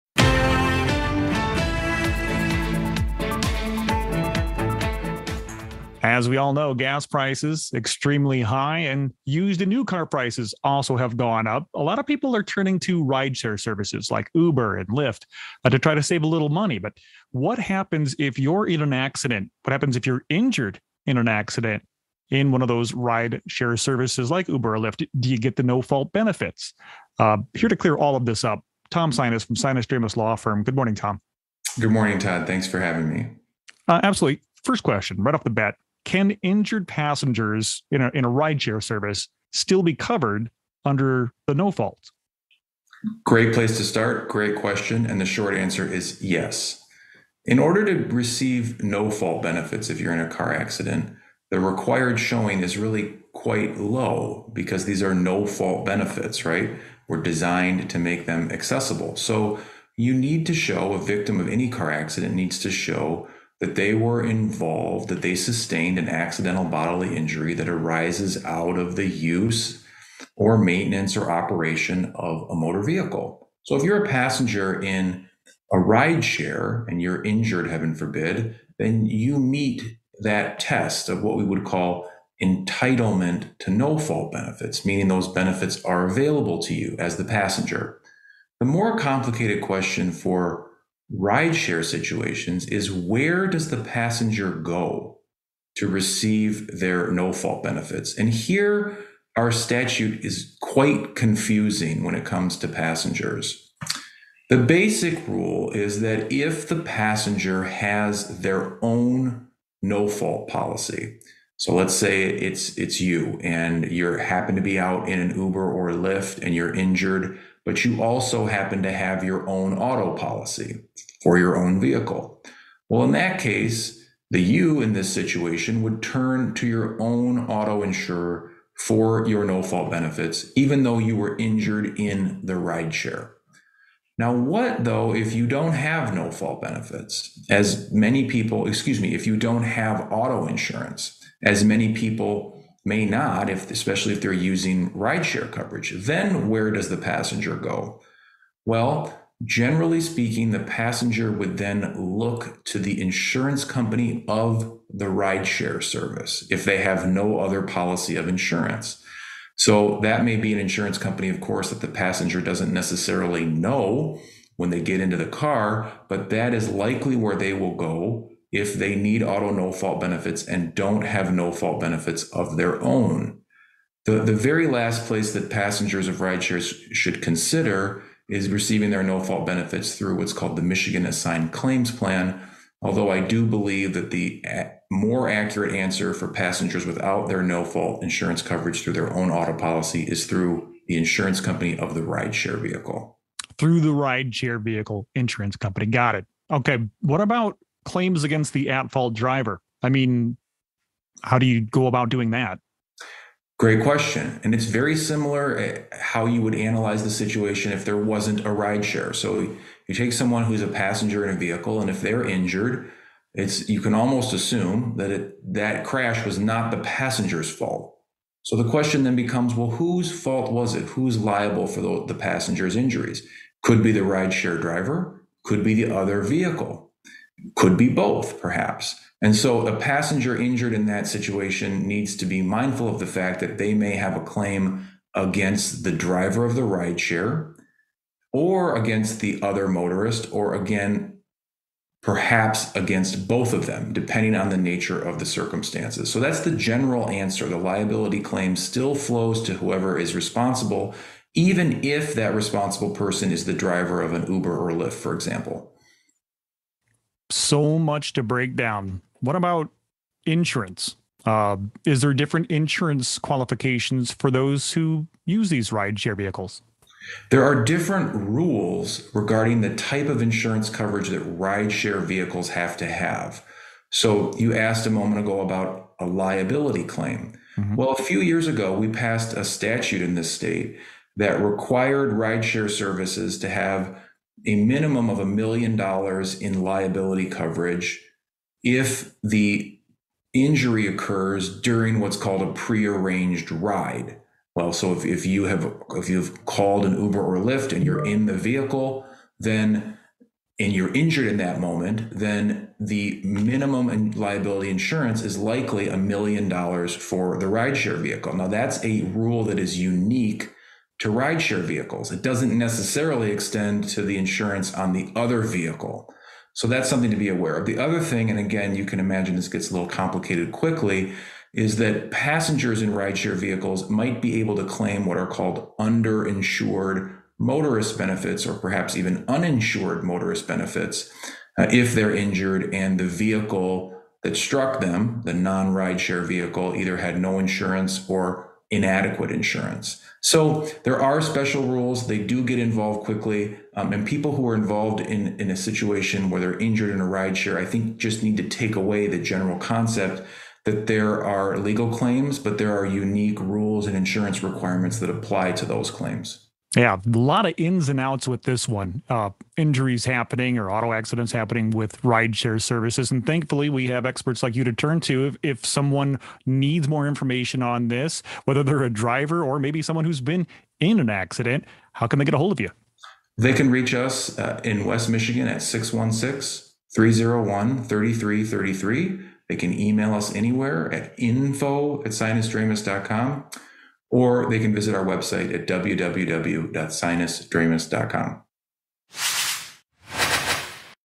Know the Law: Rideshares & Injuries
With gas prices still high and cars as expensive as ever, many people are turning more to rideshare services like Uber and Lyft to get around. But what about your legal rights if you are injured as a passenger in a rideshare vehicle? Where do you get your no-fault benefits? What about a claim against the person who caused the crash? Michigan auto accident attorney Tom Sinas explains everything you need to know this week on Know the Law.